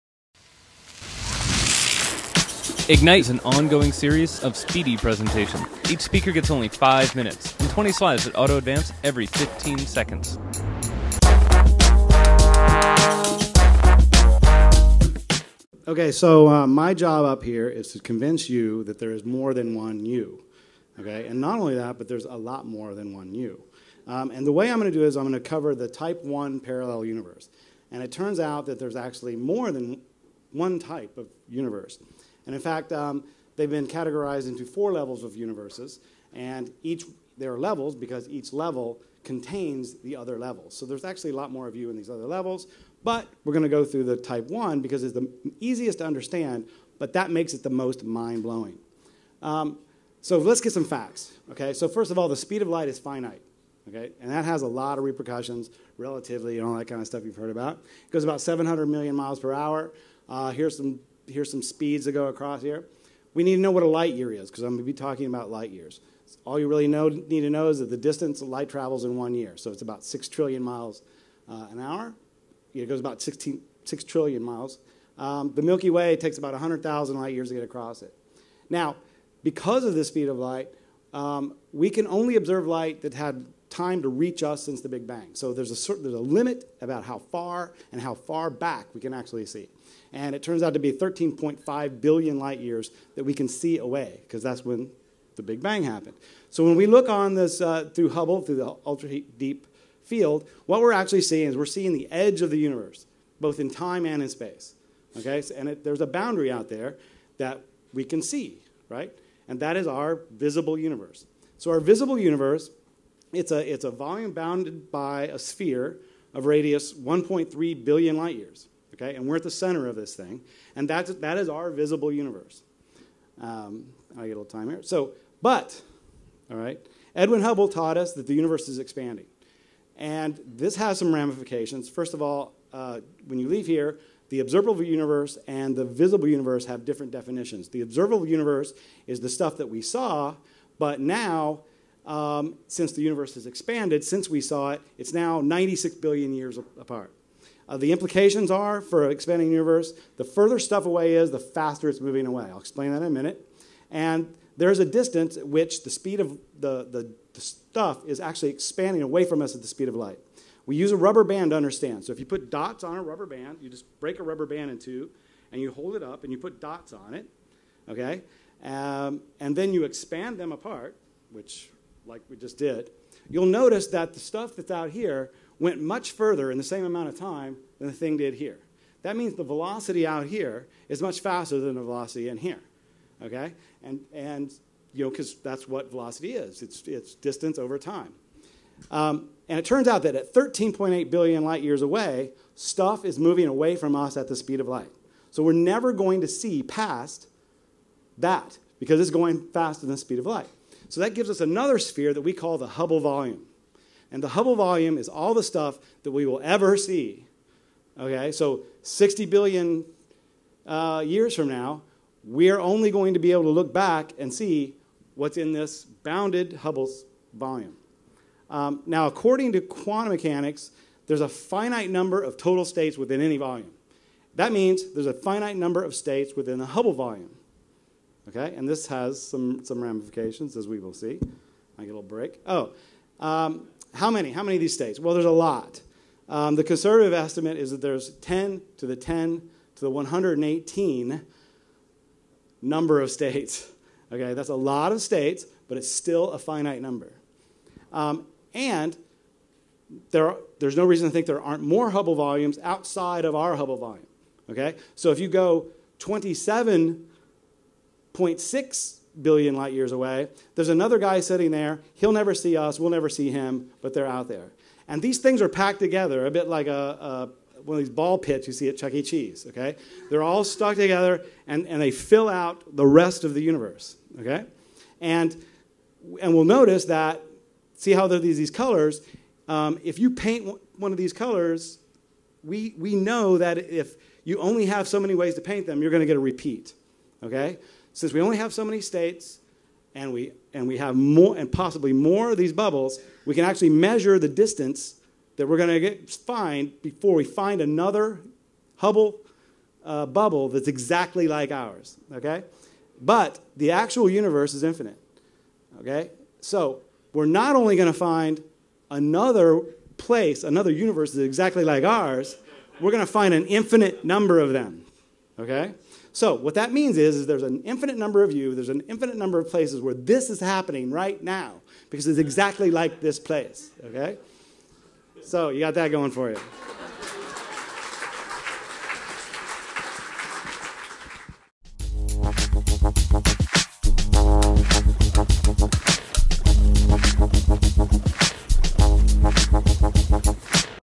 There is more than one you. The Type 1 parallel universe.
Scott Gray presents "There is more than one you. The Type 1 parallel universe." at Ignite Sebastopol 4, October 20, 2010.